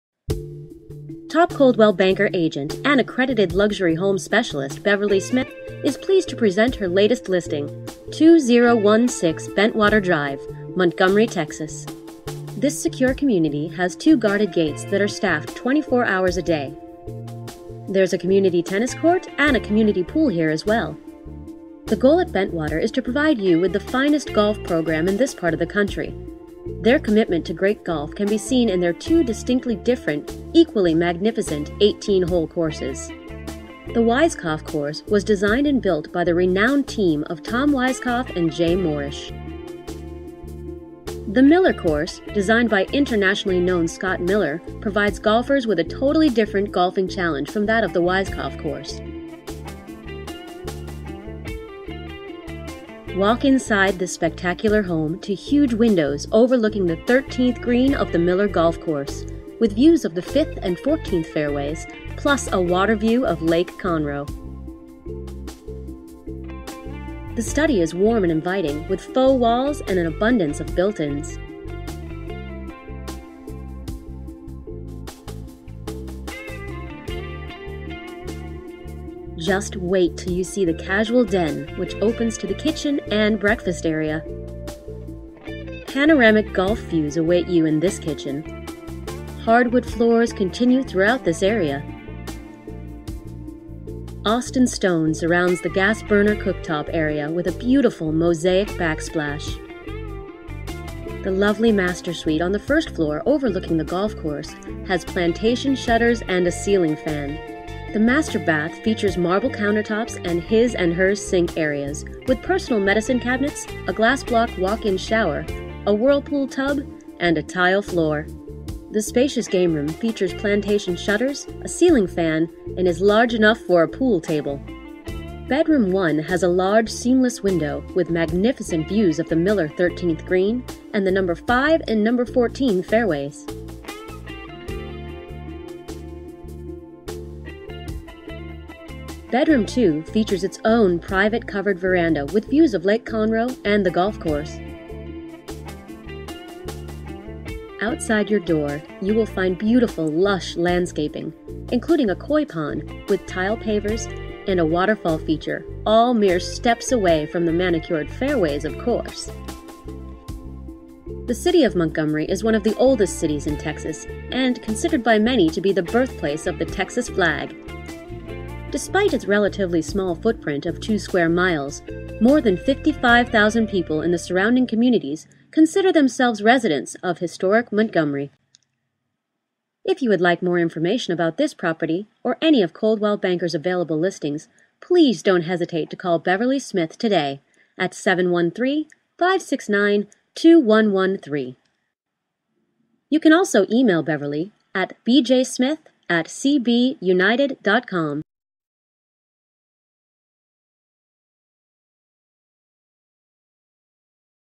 2016 Bentwater Dr, Montgomery, TX. 77356 ~ Real Estate Cinematic Video
Cinematic video of this spectacular home situated on the 13th fairway of the Miller Course with dramatic views of the 5th & 14th fairways when you first walk in plus waterview of Lake Conroe. Large rooms through out with lots of built-ins for storage. Commerical butted glass windows along back of home for incredible views.Wet bar in family room with gas fireplace open to kitchen.Large master suite with his & hers closets & sinks. Study down, 3 bedrooms up with game room. High ceilings & hardwood floors,